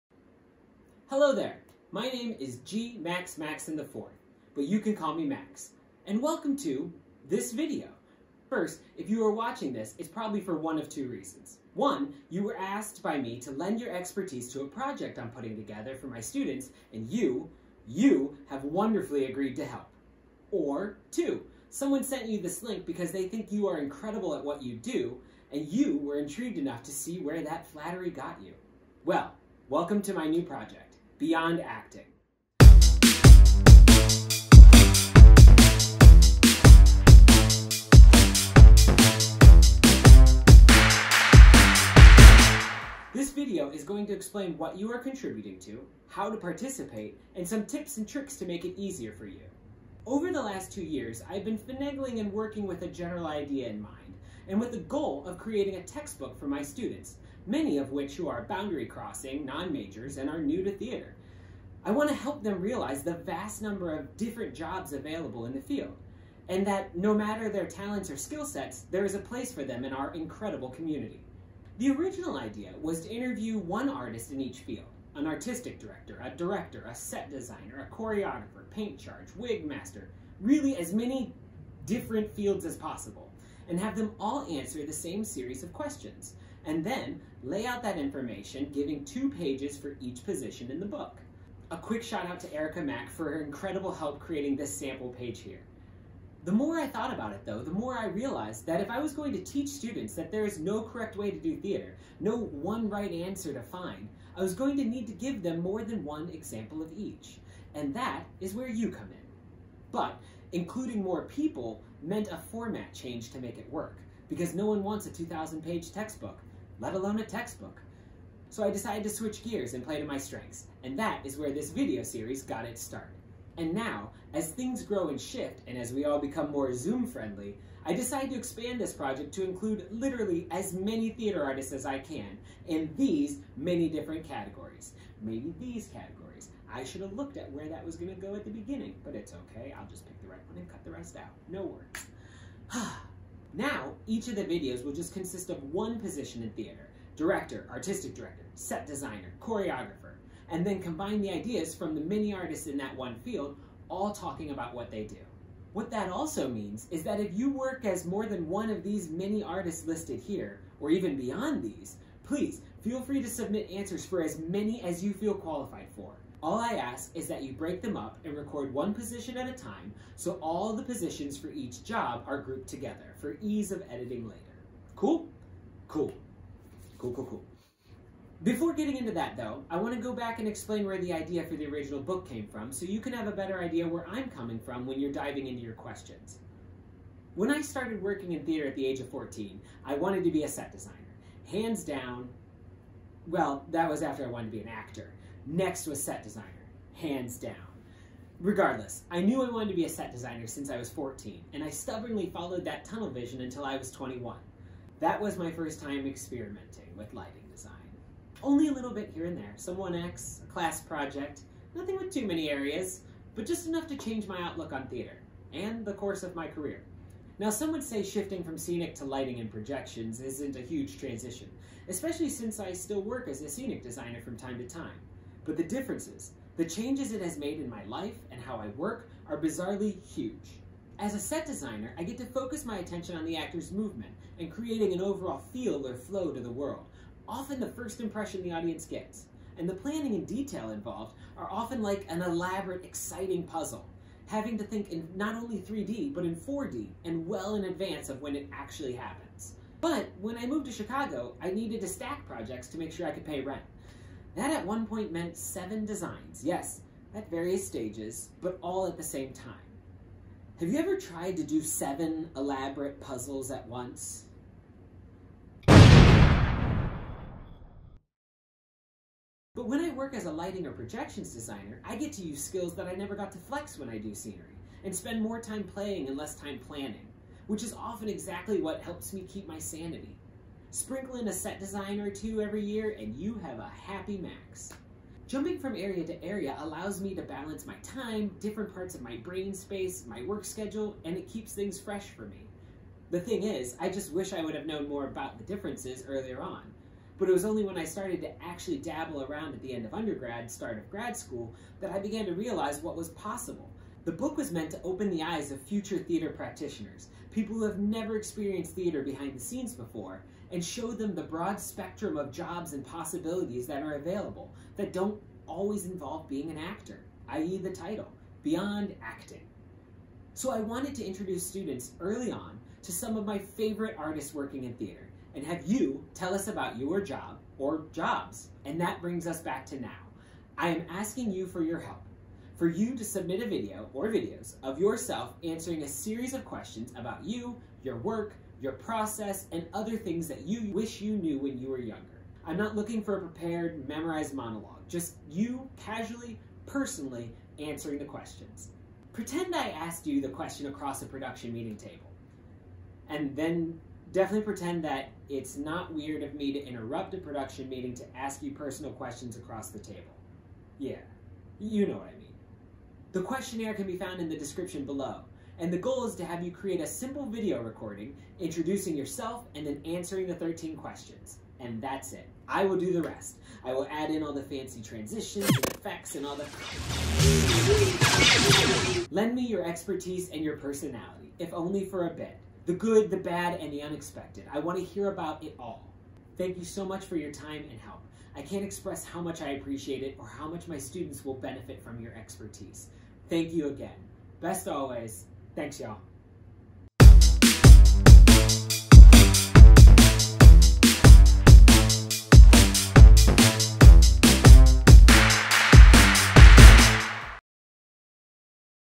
Beyond Acting: Artist Request Video
Are you a Theatre Artist interested in getting involved in the Beyond Acting Video Series?
Full Details: ivMaxin.com/ba

ivRealProductions